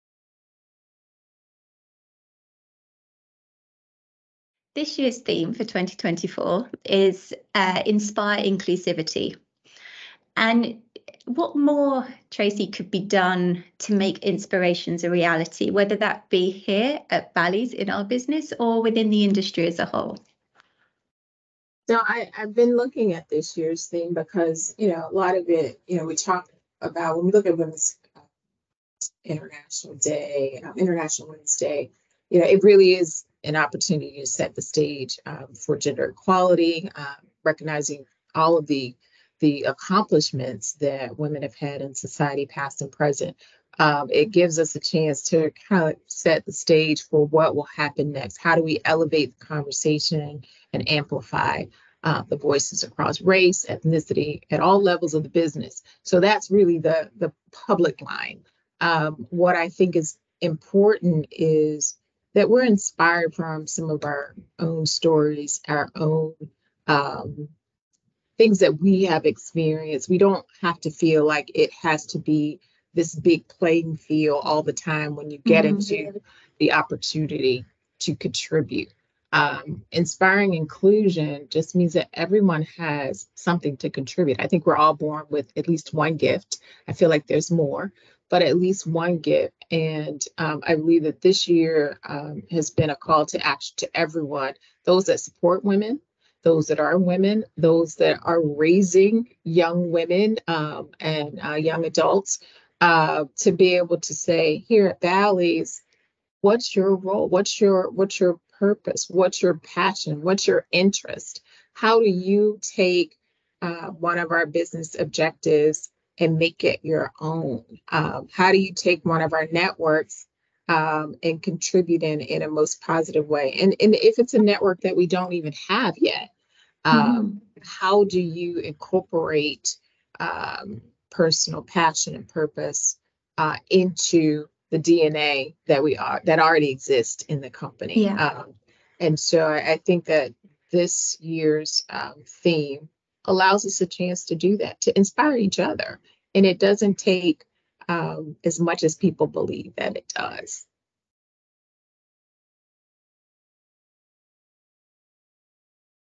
IWD
🌟 Happy International Women's Day 🌟 Join us in celebrating the incredible women who play an integral part in the Ballyverse, and whose dedication, tenacity, and passion inspire us every day.

While every corner of our organization will celebrate in its own unique way, we were all thrilled with the opportunity to hear from our very own Tracey Wiley in a GLOW network event. As Executive Director of Diversity, Equity, and Inclusion, Tracey gave us some profound insights into her role and the enormous significance of Inspiring Inclusion.

Today is a day to remember all the remarkable women who have shaped our lives, careers, and communities. Let’s keep working together to break down barriers, defy stereotypes, and inspire a more inclusive future! 💪💖